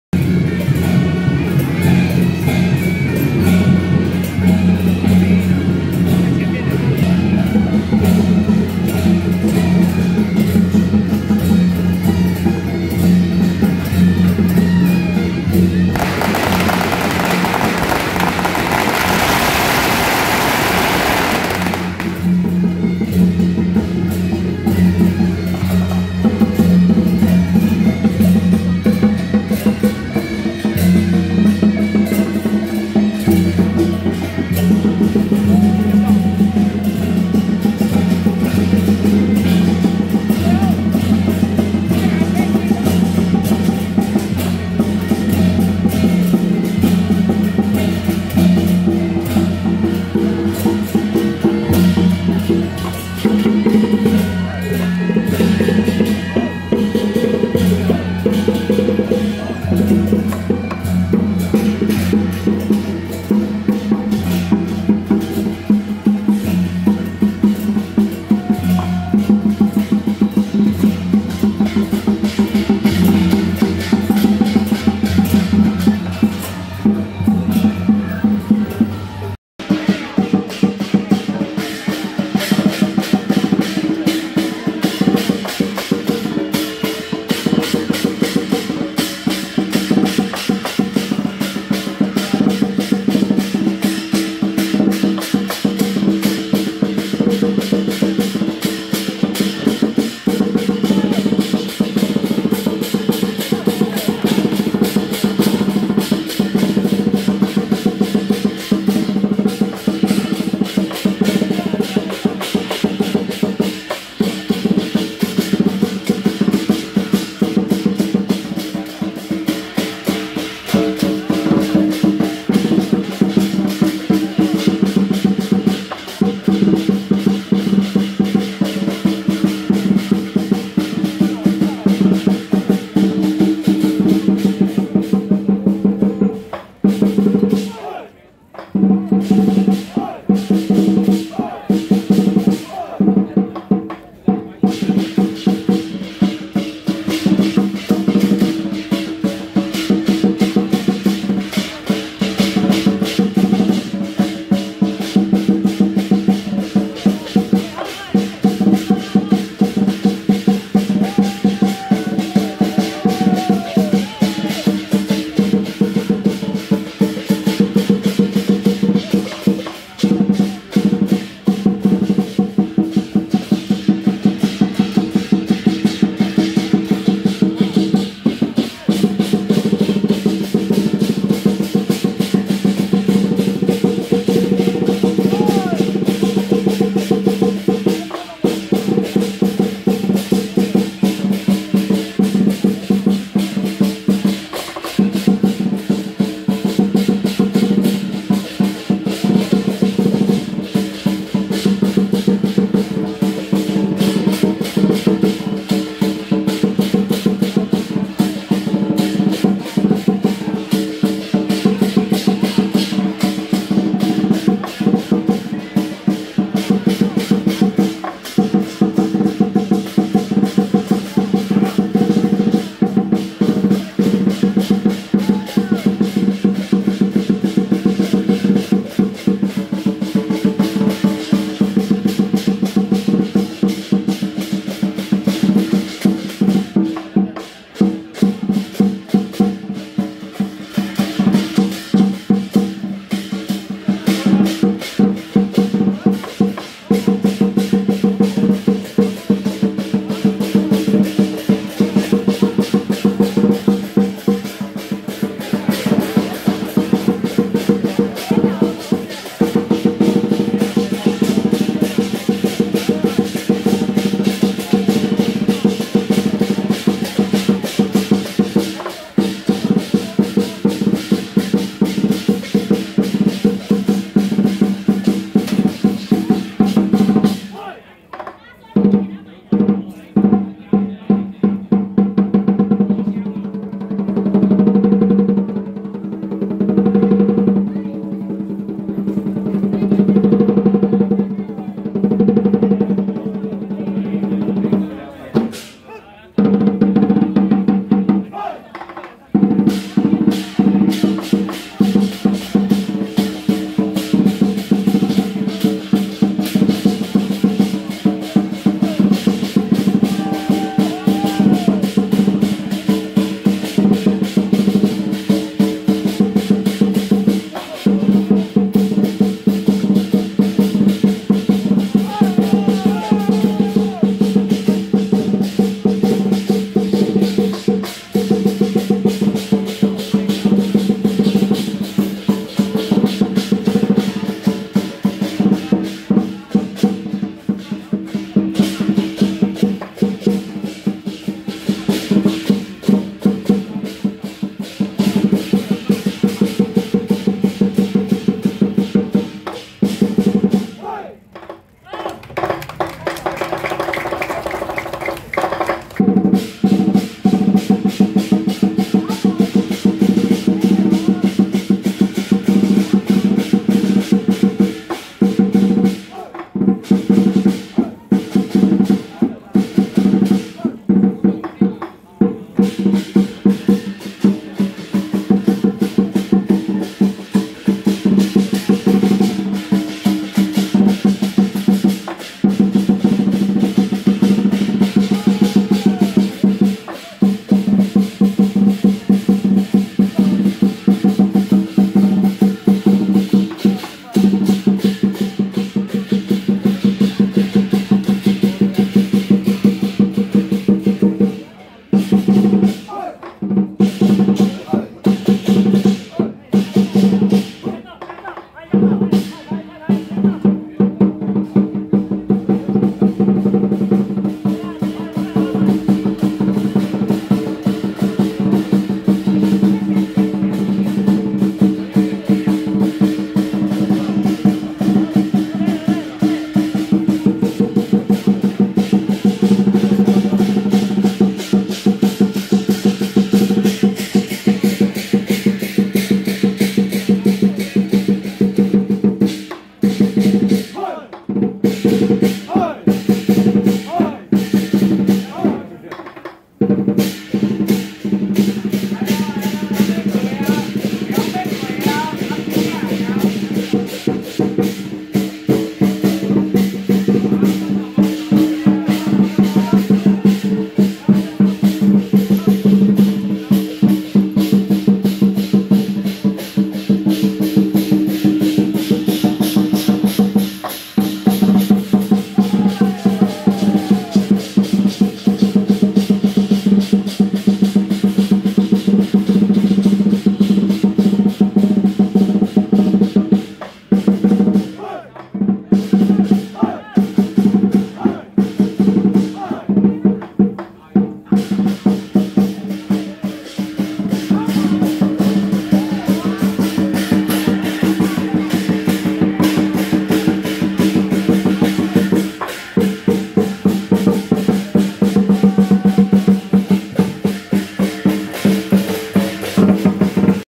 【台北威聖舘-技術龍】~111台北田寮媽祖會恭迎天上聖母35週年遶境
台北威聖舘-技術龍(0:00)過境 北門口保正殿 (1:20)入 台北田寮媽祖會 紅壇